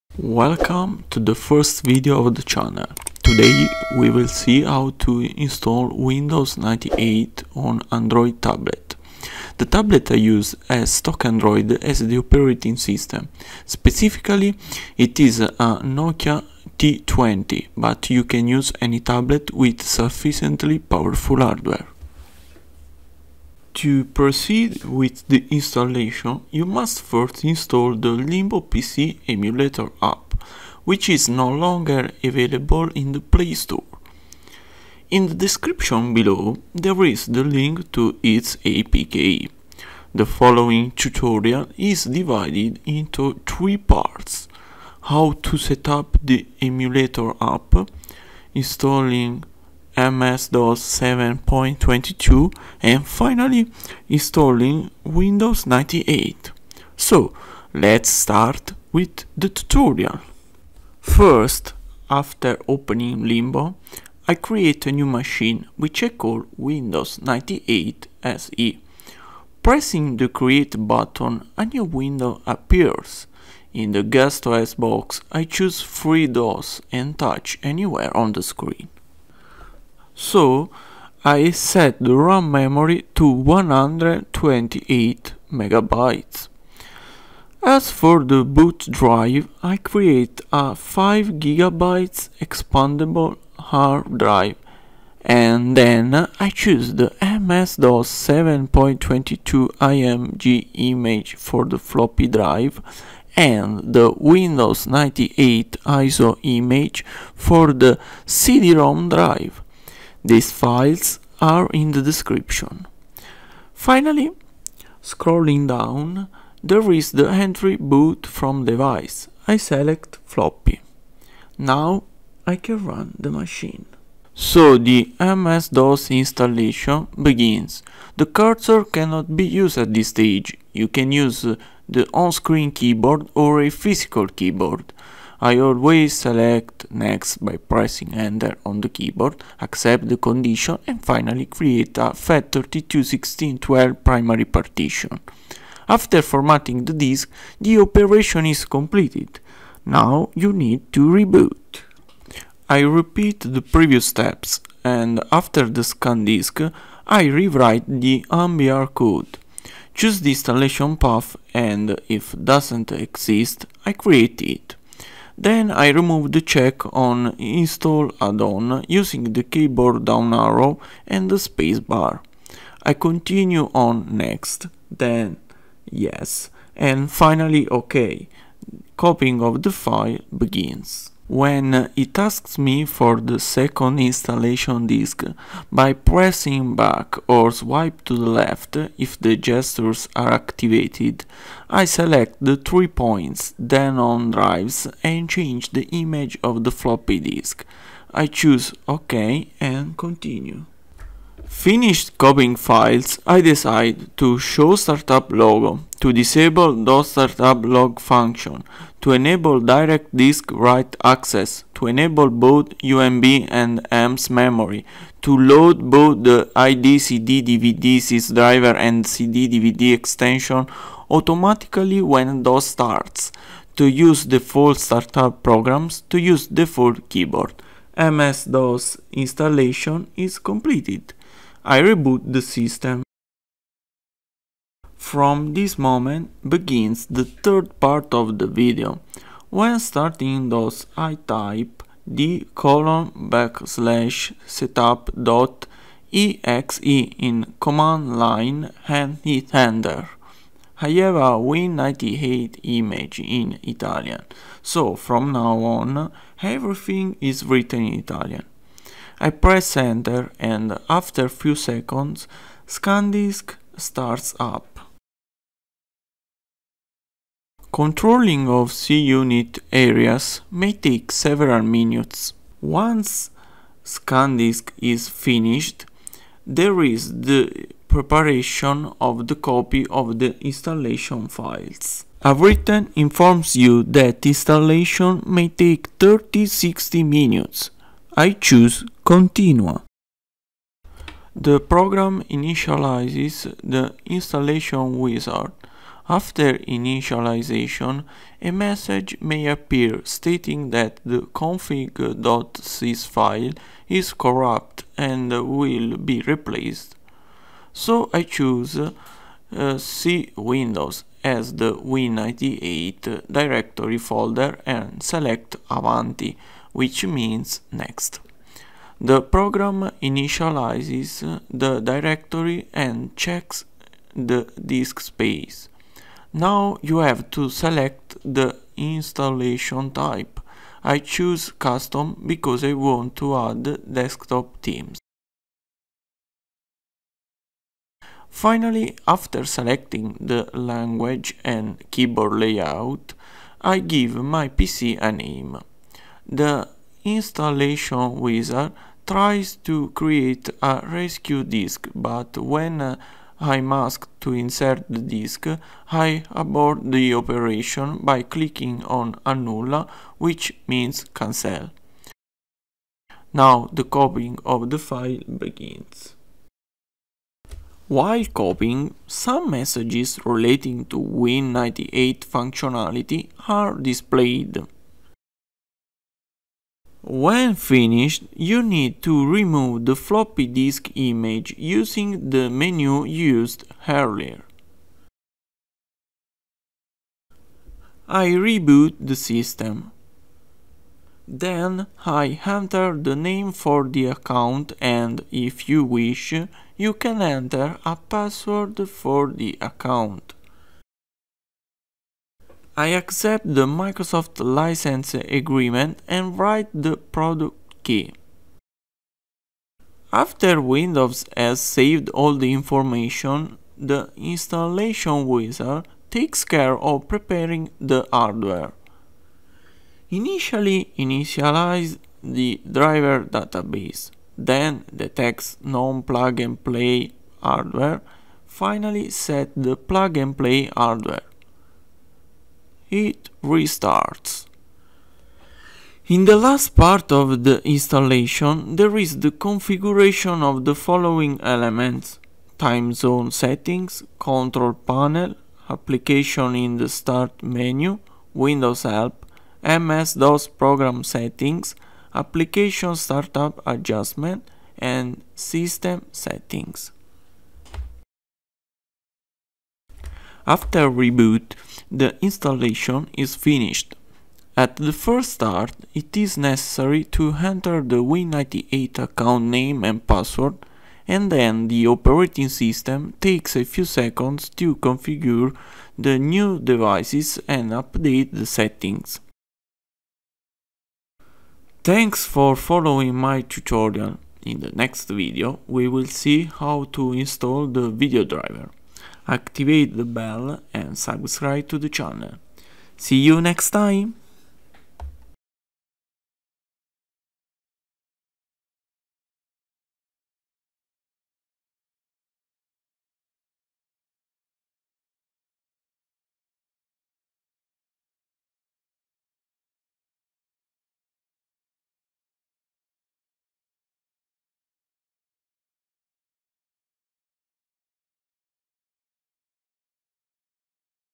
How to install Windows 98 on Android
In this video is explained how to install Windows 98 on Android OS using a virtual machine. It is recommended to use a device with a powerful hardware. Also, there are english and italian subtitles.

Windows 98 product key: VGVXJ-8KTCC-4V4V7-BMBC3-PXCH6

00:00 Intro
01:00 Setting up the emulator app
02:04 Installing MS-DOS 7.10
03:57 Installing Windows 98
07:37 First boot of Windows 98